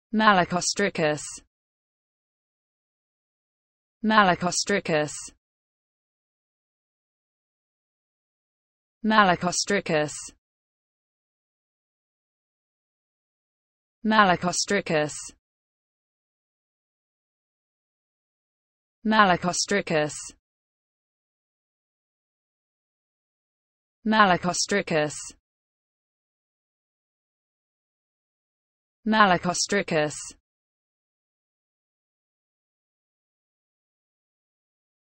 the correct pronunciation of malfunctioning in English.
This video shows you how to pronunciation malfunctioning in English.